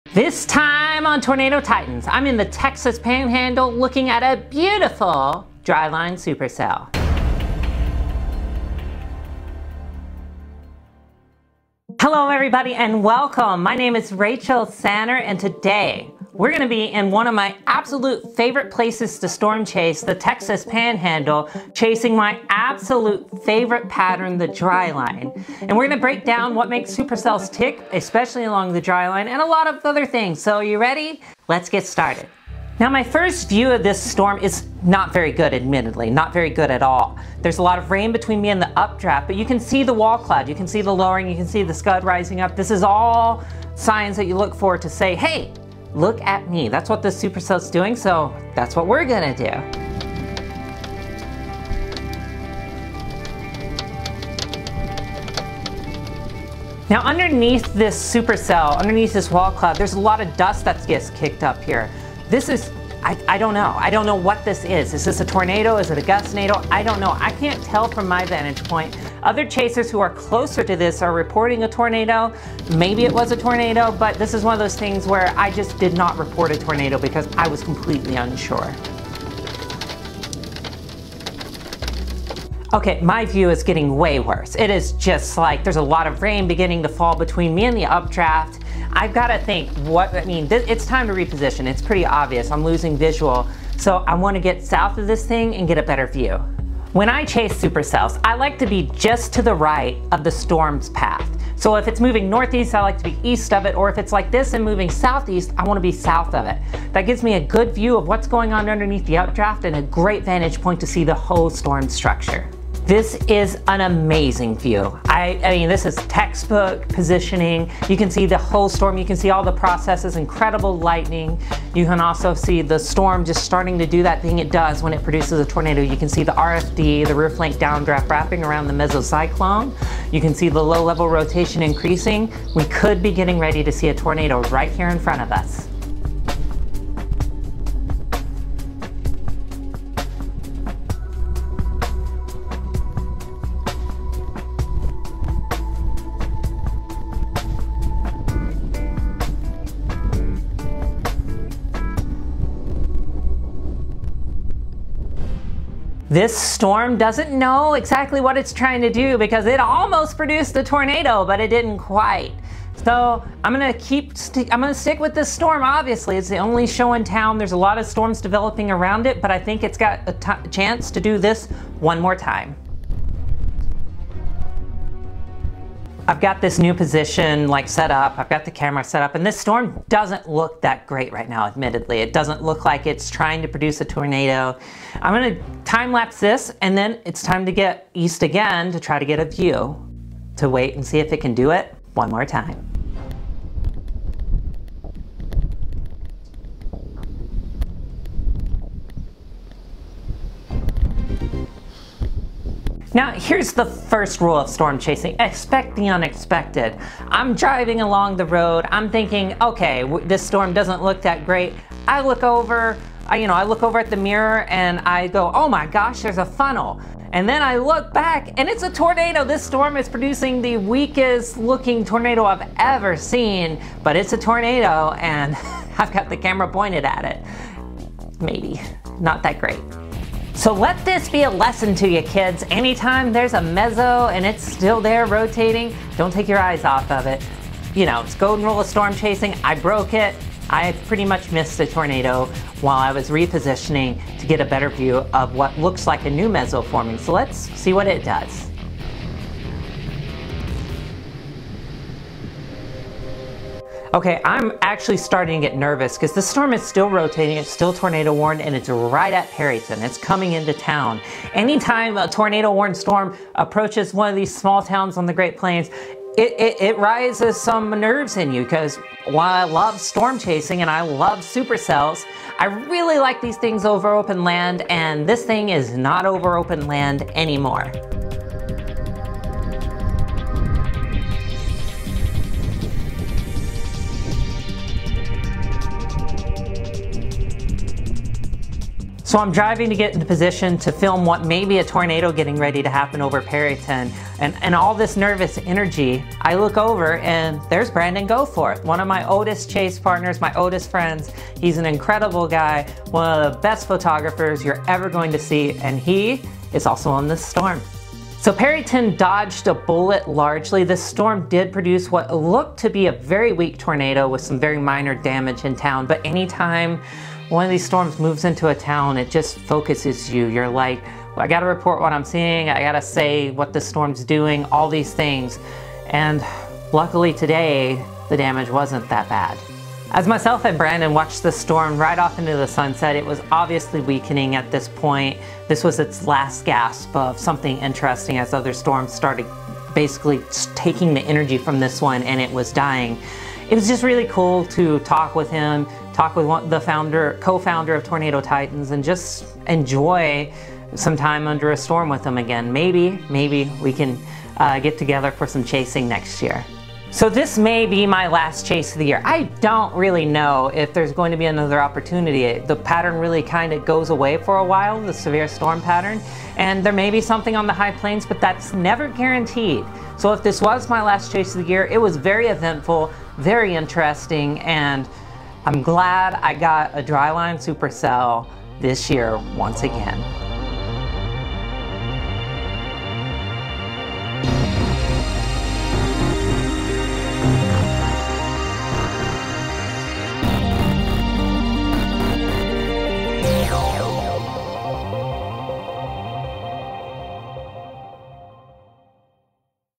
Texas Panhandle Dryline Supercell and Tornado! - May 26, 2021 Storm Chase
There weren't many traditional dryline setups on the menu for me this year, so when one finally matched up with my schedule on May 26, I had to give it a go.

My initial target was the Oklahoma Panhandle with the thought I might need to hedge into Kansas as the day went on -- but looking over surface obs and satellite imagery it was obvious that the storm of the day in my target zone was definitely going to be in Texas.

After a not so daring core punch, I found myself in position as this storm ramped up and began a weak tornadofest. This was a good quality dryline storm, check it out by watching the vid!

0:00-0:32 Intro
0:33-1:35 My First View
1:36-2:35 Classic Storm Structure
2:36-3:21 Now Higher Based
3:22-4:13 Unexpected Tornado
4:14-5:57 Storm Ramps Up Over Perryton One Last Time
5:58-6:40 Conclusion